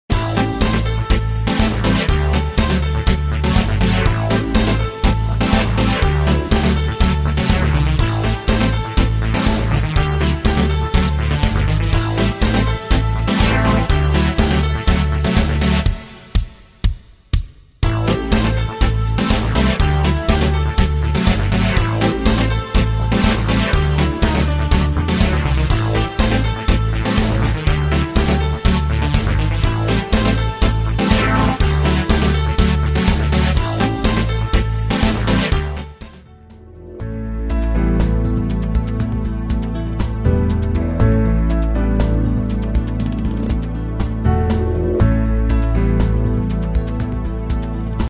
Watch Golden Retriever Video from Silver Oak Casino
It’s a dog's life in this fun and action packed slot machine.  Look for the diamond encrusted bones to get to the jackpot.